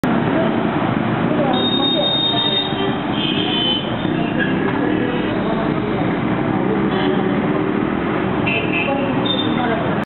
Station Road la nuit
Une des rues principales de Kolhapur de nuit, un chien traverse la rue, les motos passent